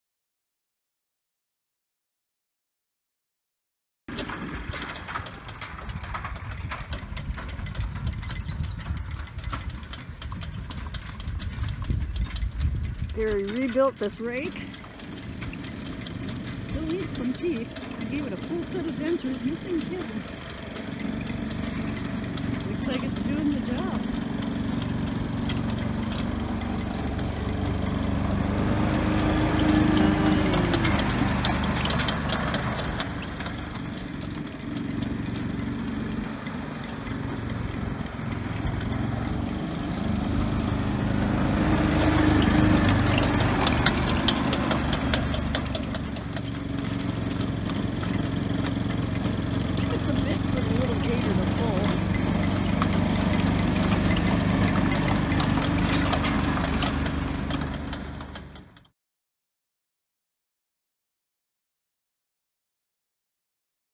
Hay Rake Gets New Life
Hay rake is ready for the field! It was toothless, had cracked gear box, body parts damaged or missing - but now it has new life.  Don't be bored, watch farm and animal videos @Trulylindahl